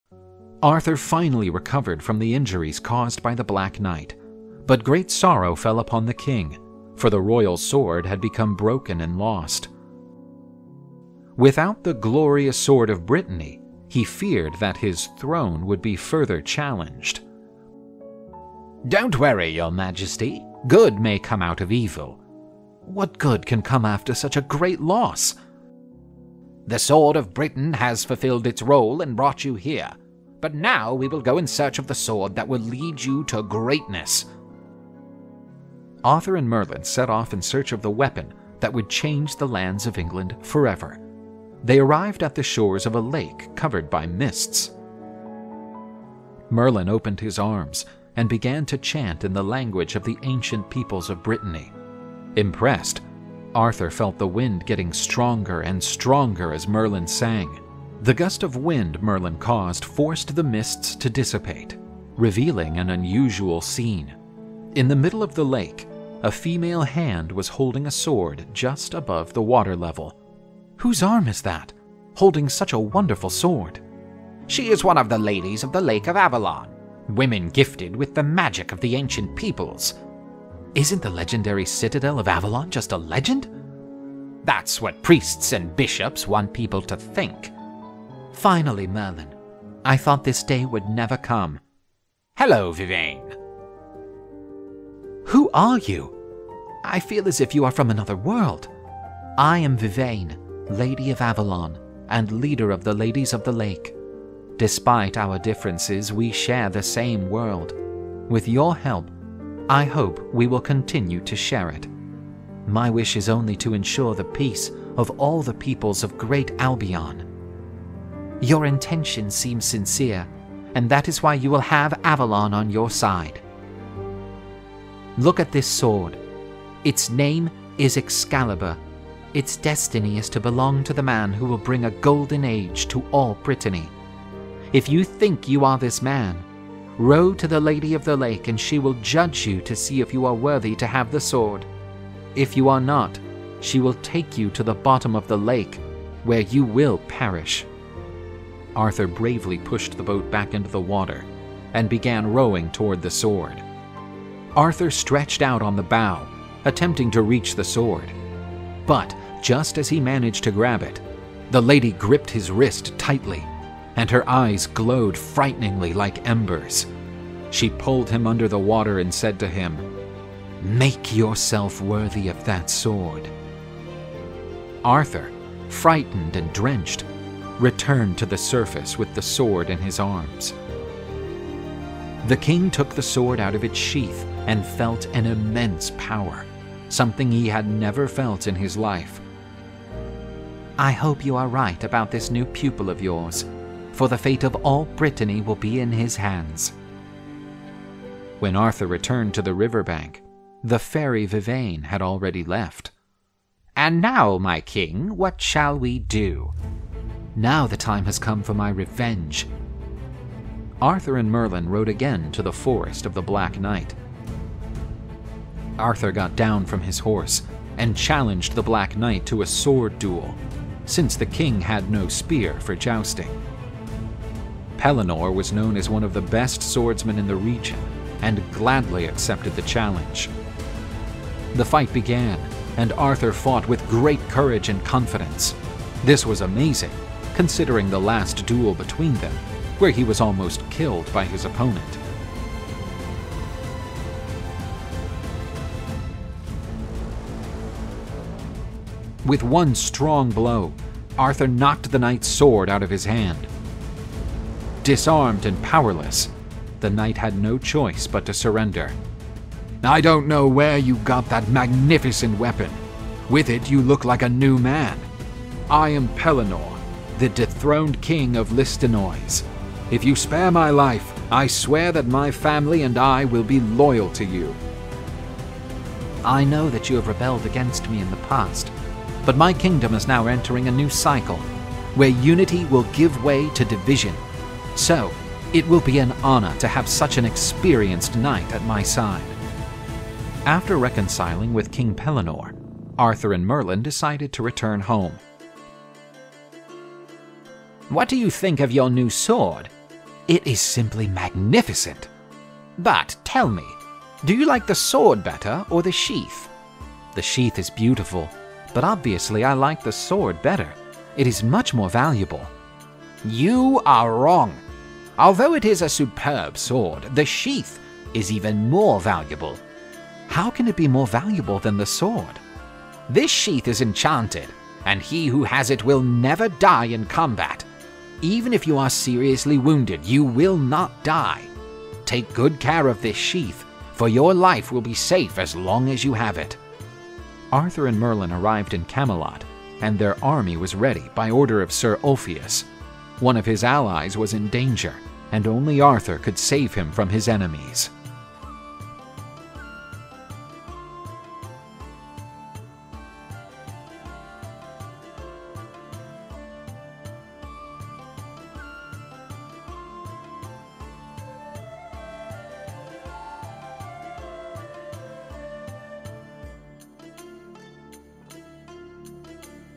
Excalibur: How king Arthur Gained his Glorious Sword - Legends of Camelot #05 - See U in History
Art: Caio Filipe
Script: Bruno Viriato